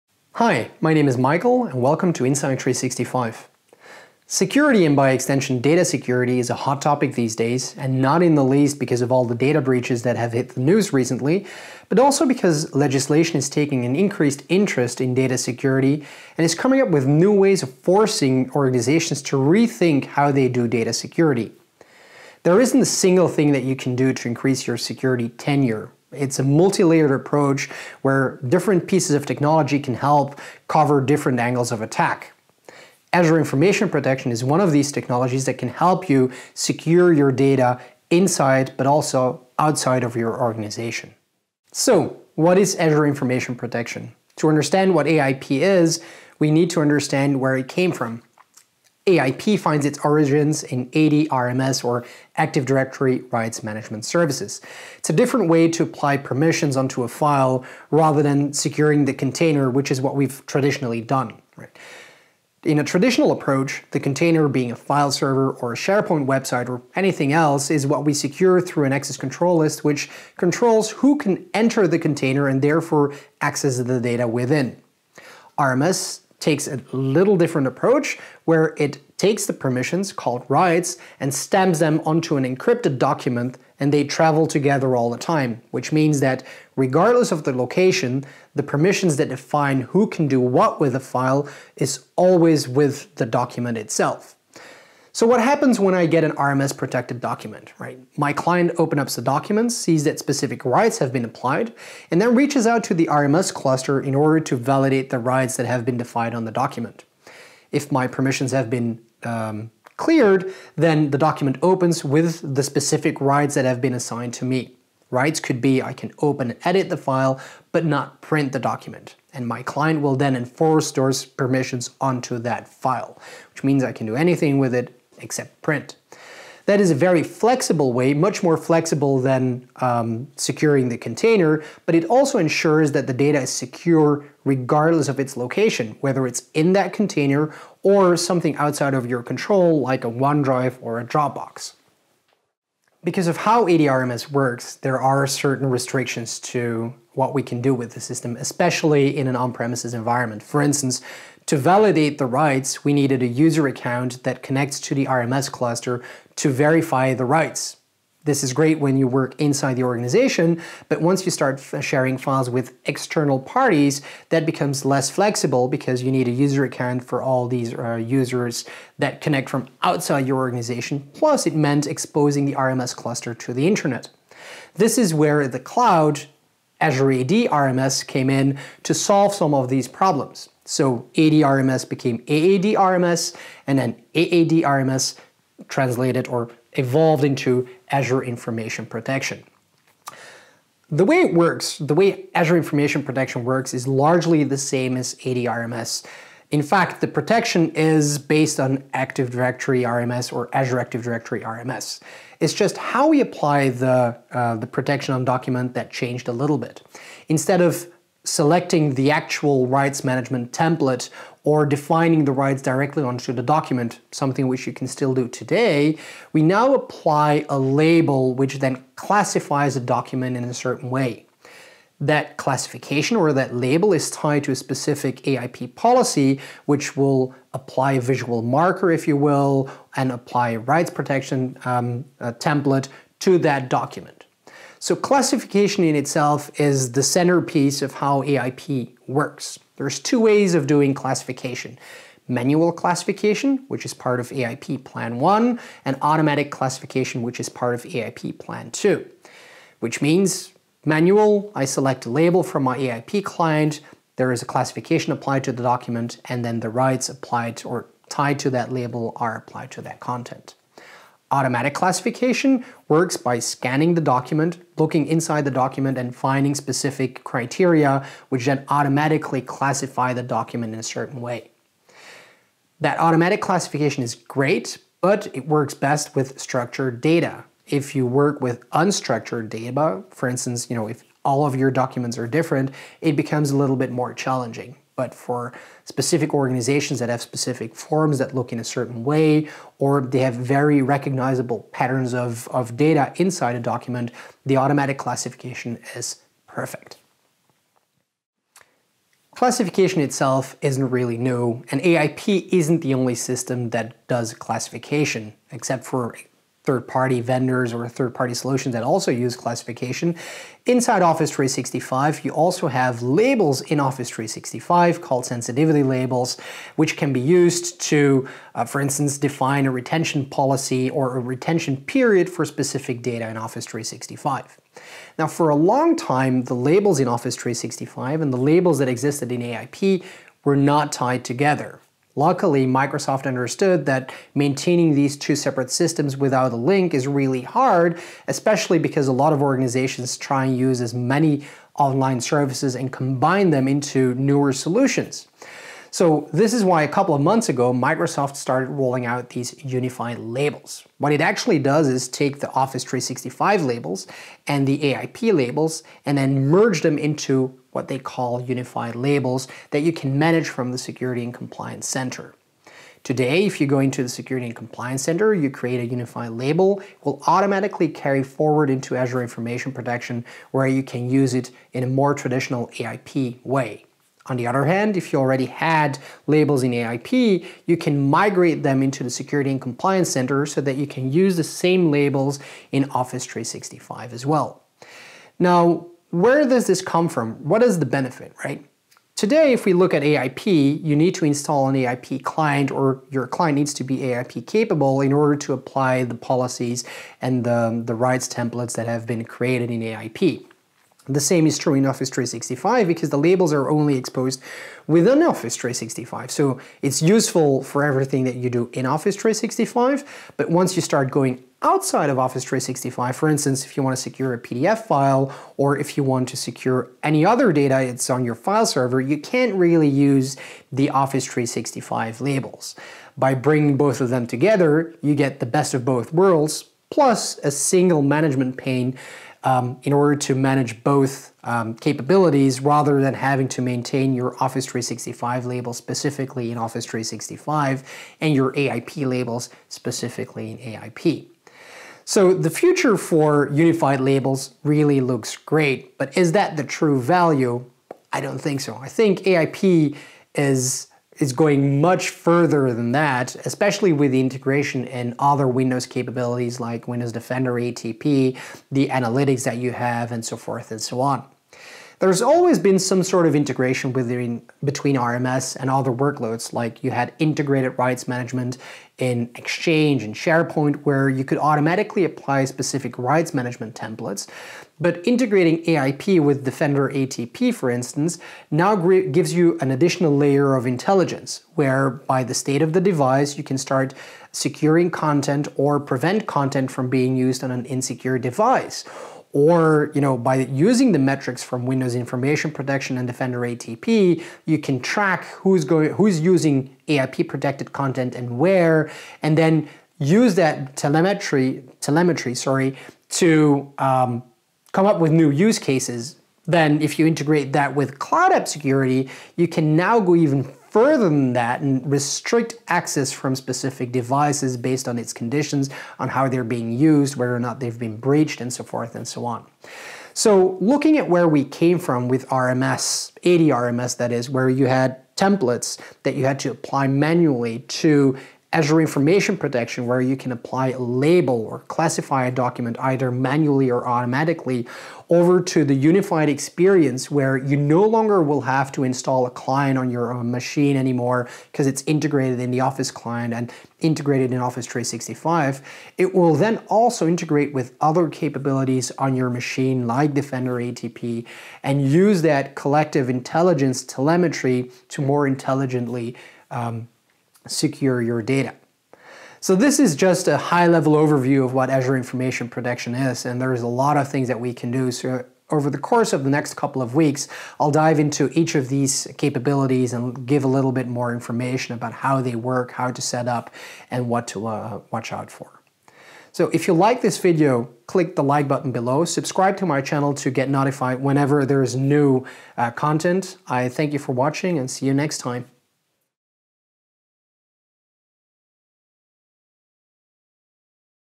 Insight 365 Ep. 1 - Introduction to Azure Information Protection
In this first video I discuss what Azure Information Protection is, how it works and why it can be useful in your DLP and Data Protection strategy.